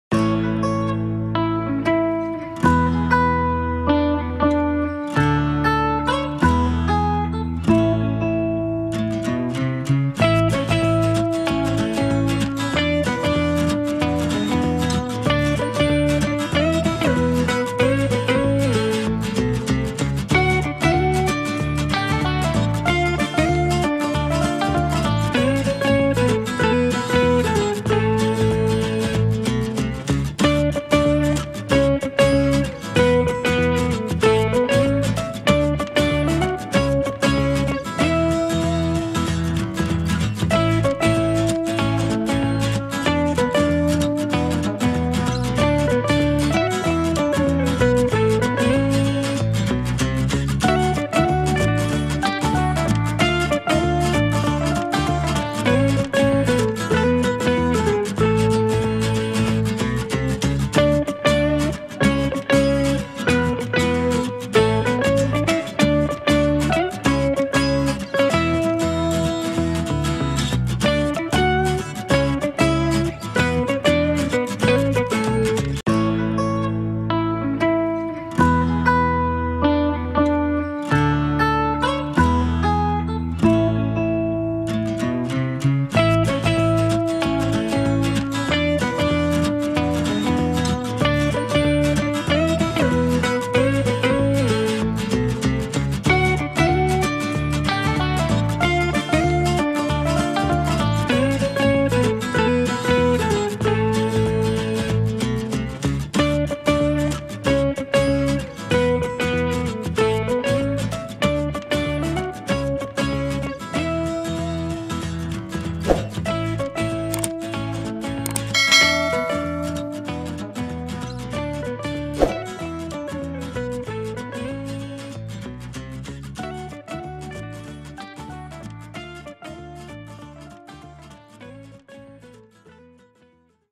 Chicken family Drawing, Painting and Coloring for Kids, Toddlers Easy Drawing
Join us on an egg-citing adventure as we bring a charming chicken family to life on the canvas. From fluffy chicks to proud roosters, every member of the flock is waiting for your artistic touch to make them shine. 🌟 With easy drawing techniques designed for little hands, even the youngest artists can join in the fun and unleash their creativity.

Your support means the world to us and helps our channel grow. 🙏👍 Let's keep the creativity hatching! 🎉